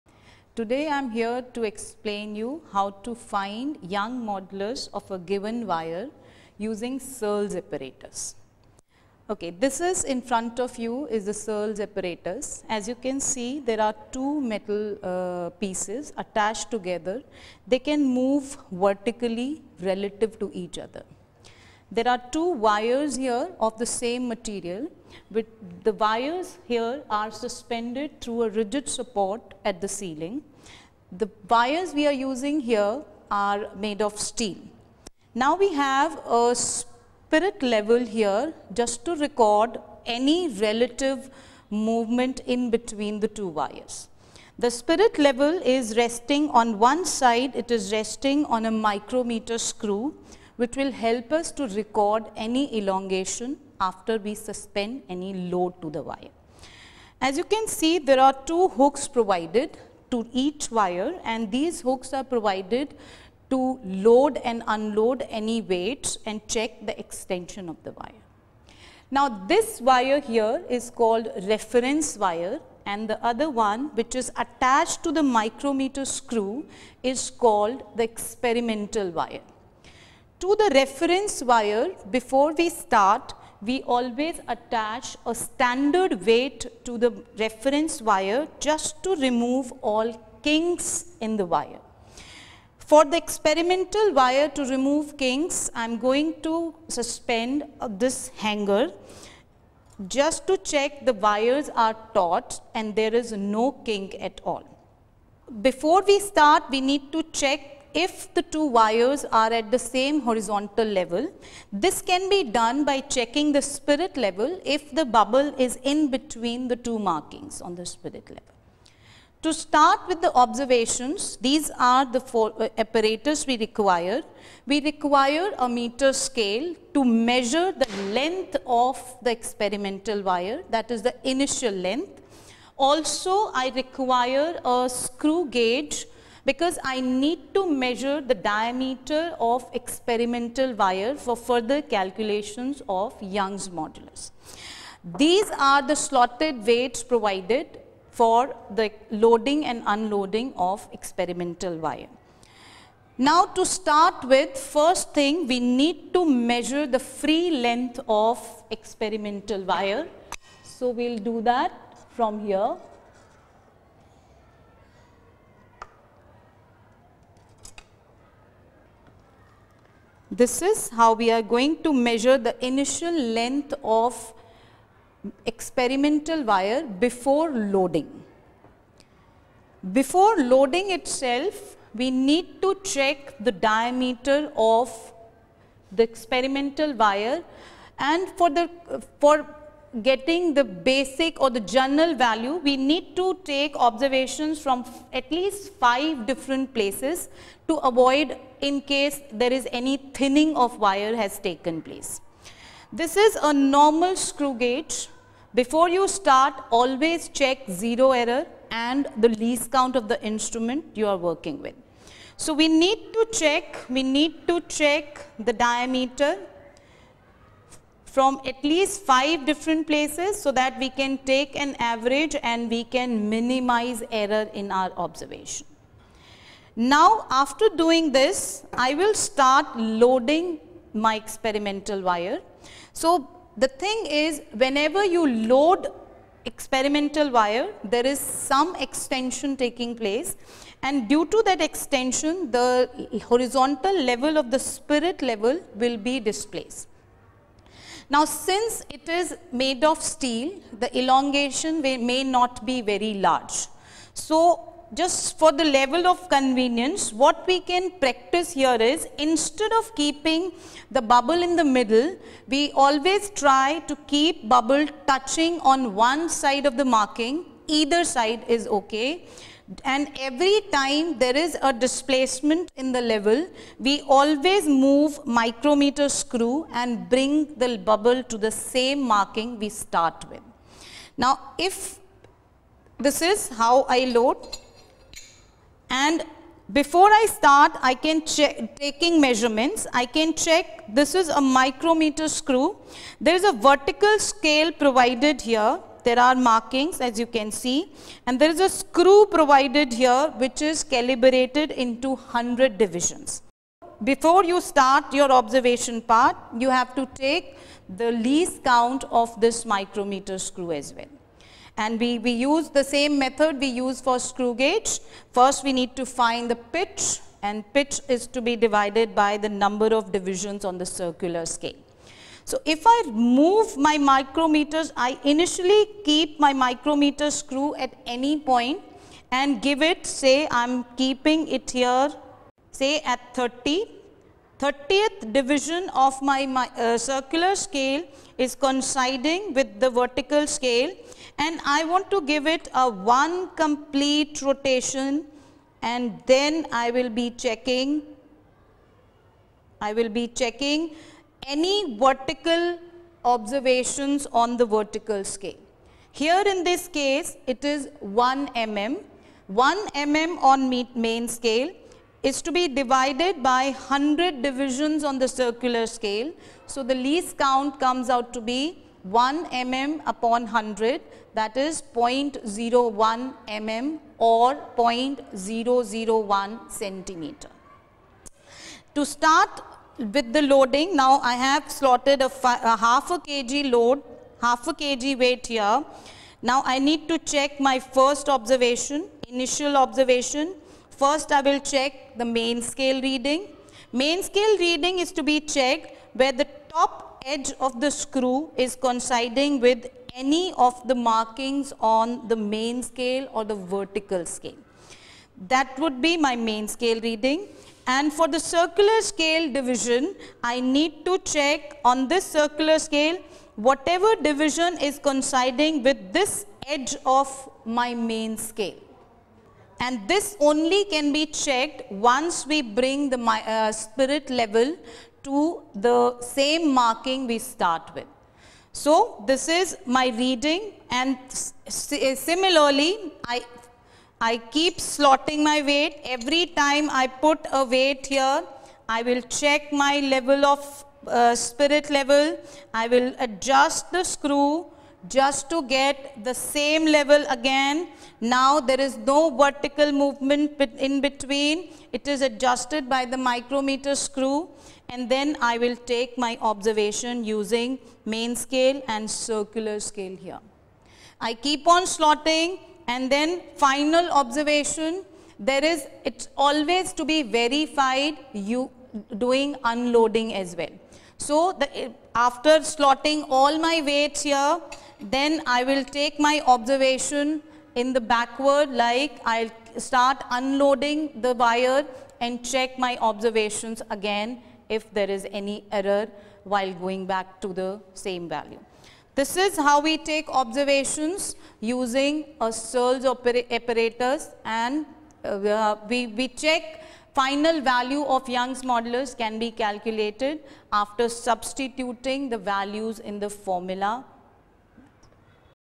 Searle's Apparatus
An experiment to find the young's modulus of given wire using searle's apparatus.